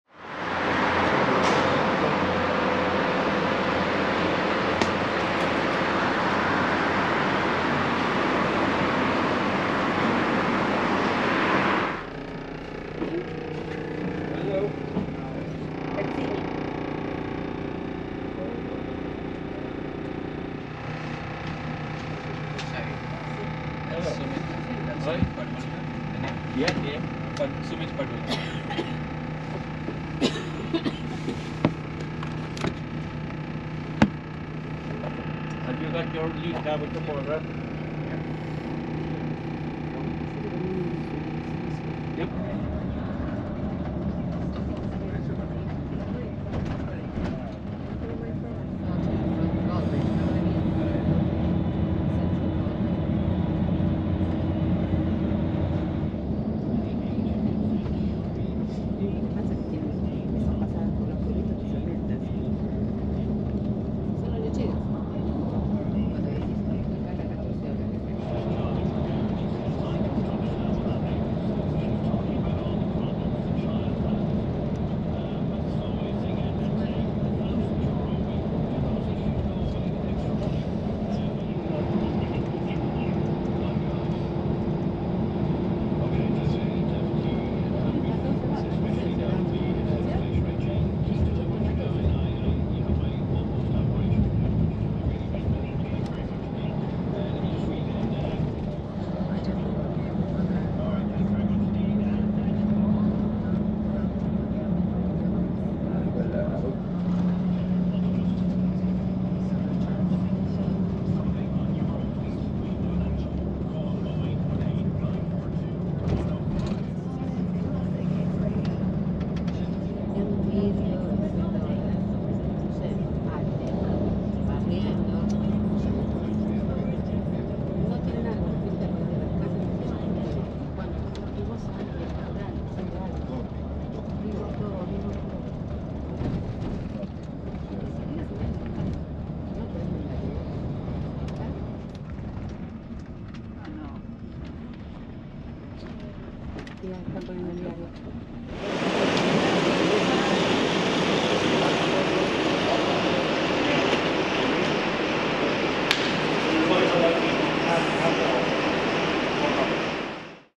Cork to Galway CityLink Bus | Ireland
Taking the CityLink bus from Cork to Galway, Ireland.
Cost: €22
Length of Trip: 3 hours